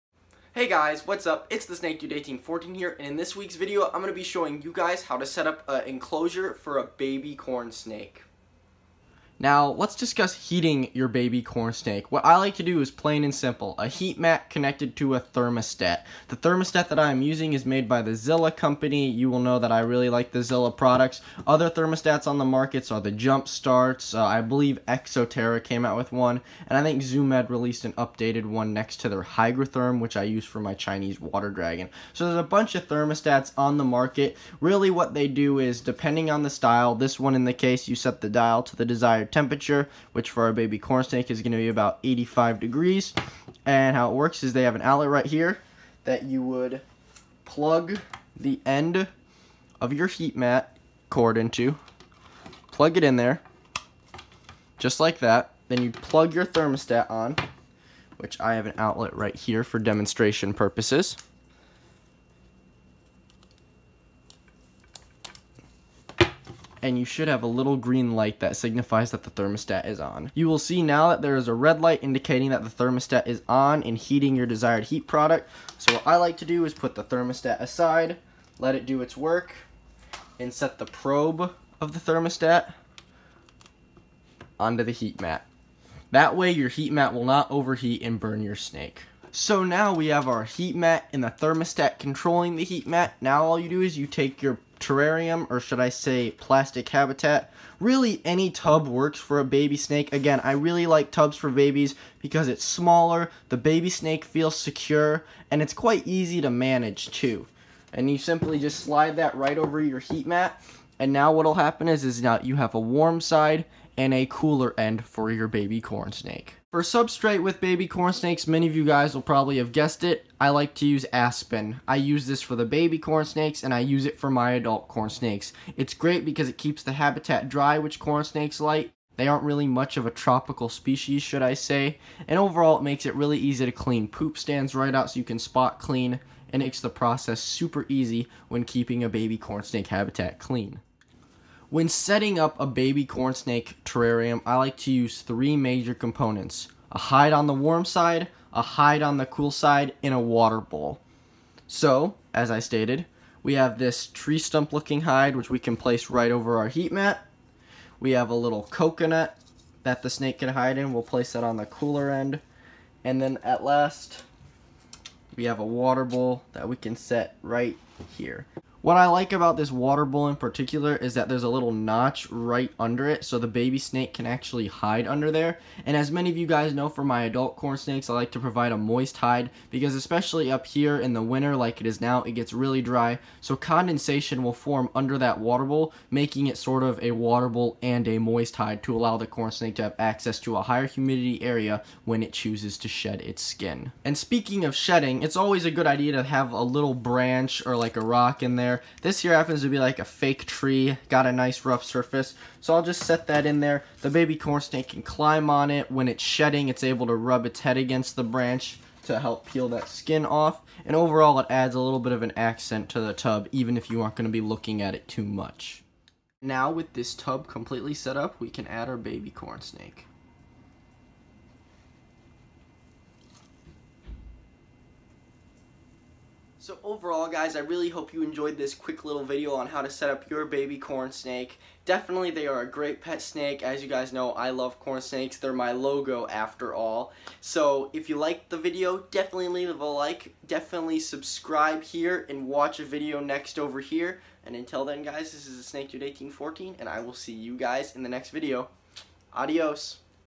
How To Setup a Hatchling Corn Snake.
Thank you all so much for watching! I seriously hope you found this video helpful in terms of setting up your hatching corn snakes! They really are great first pets and truly make wonderful additions to new and experienced keepers alike.

If you have any questions about the setup that I did not answer in the video!

Check out other Videos in Playlists! Below!